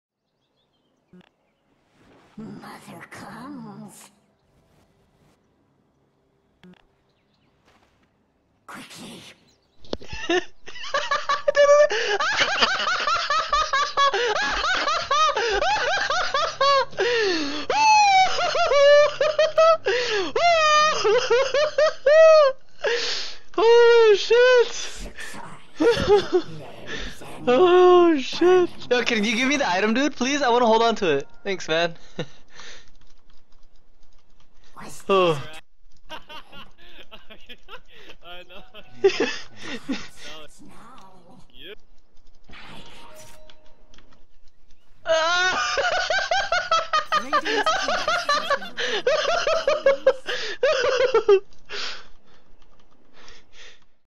DeMoN's Hysterical Reaction to Invoker's Unfortunate Luck
Justin (Invoker) decides to taunt Demon by dropping his null talisman. However Invoker is harshly punished as he loses his nulls, as well as gets his courier sniped. Demon is amused.